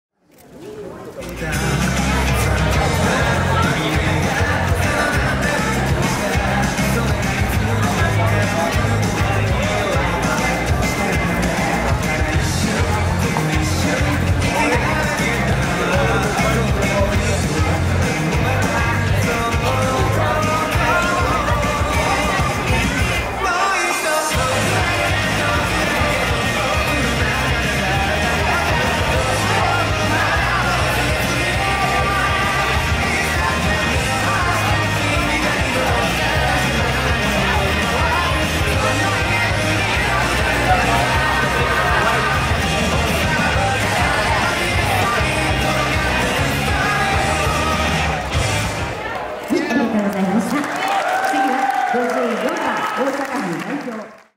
2018日本ジュニアボディビル選手権 泉風雅フリーポーズ
2018年10月7日　メルパルク大阪ホール（大阪市淀川区）
第30回日本ジュニアボディビル選手権大会
優勝　泉風雅選手（ゴールドジムウェスト東京）

予告なく動画が視聴できなくなる場合がありますことを予めご了承ください。